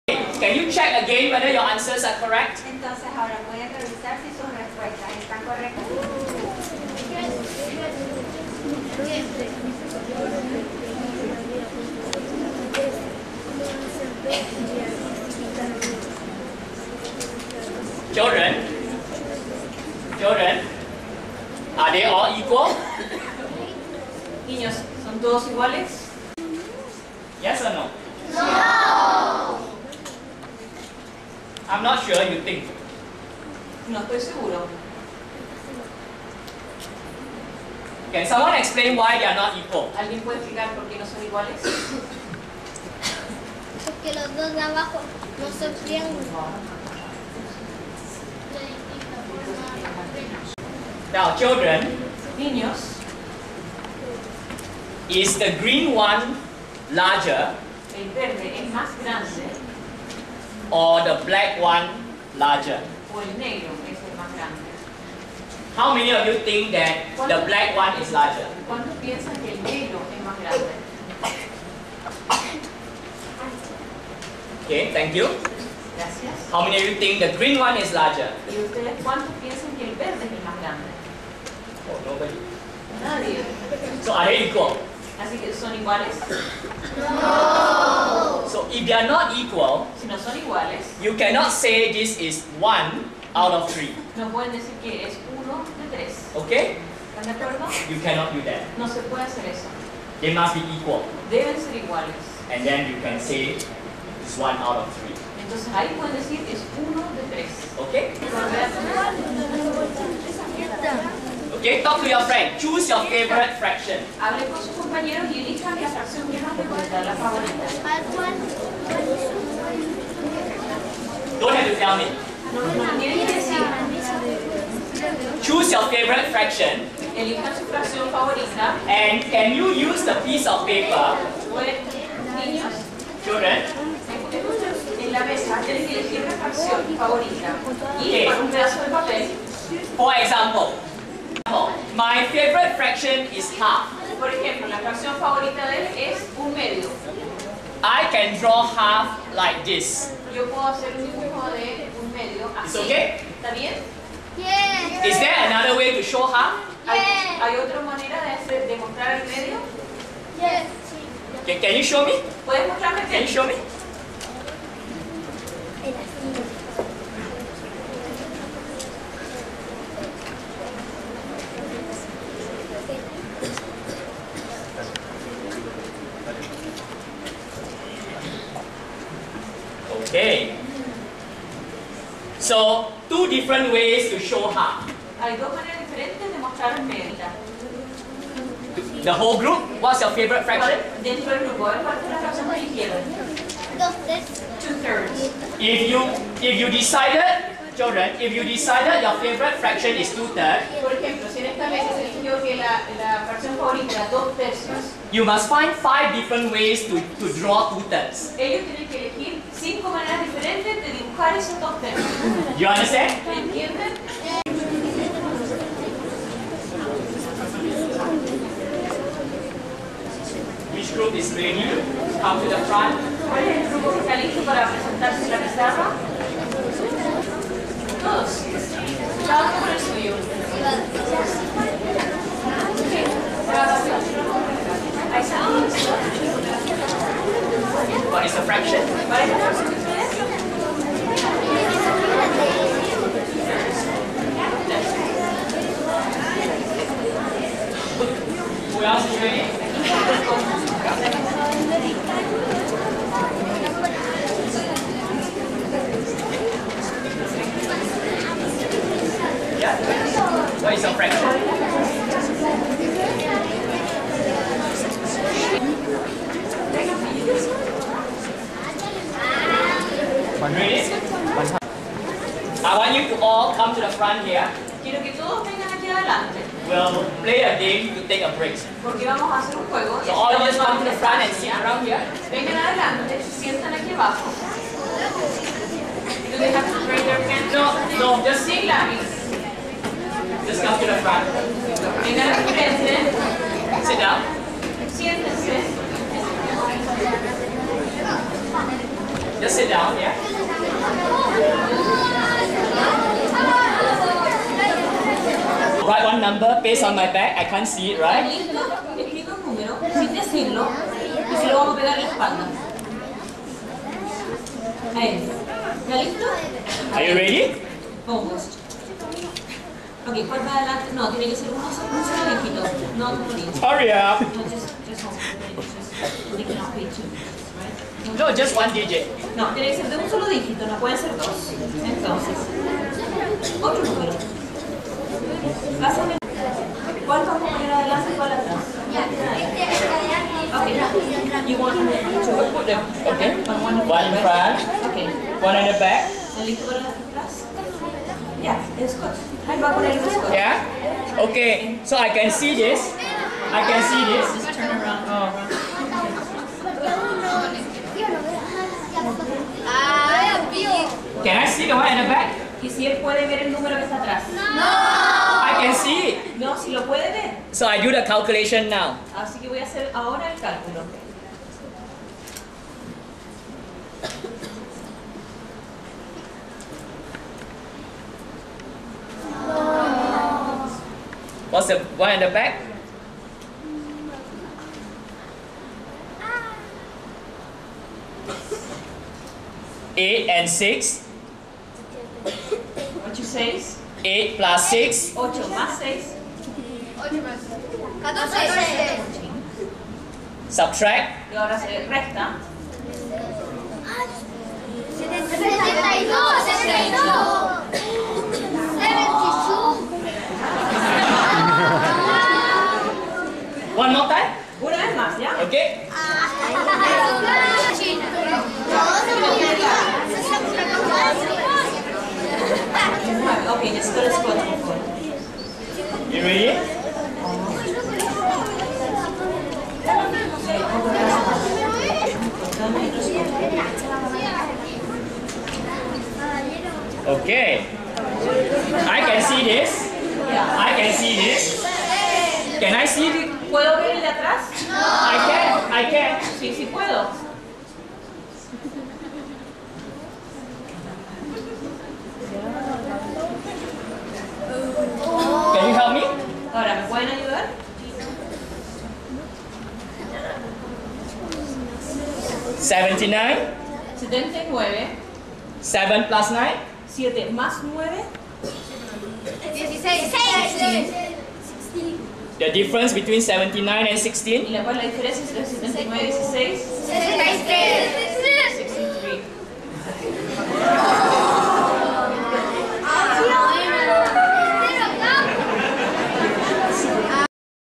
Dr. Yeap Ban Har - Singapore Math - Fracciones parte 2
La enseñanza de las fracciones con estudiantes de de 4° año de Educación Básica Colegio Arzobispo Manuel Vicuña, La Legua.
Dr. Yeap Ban Haar
Segunda parte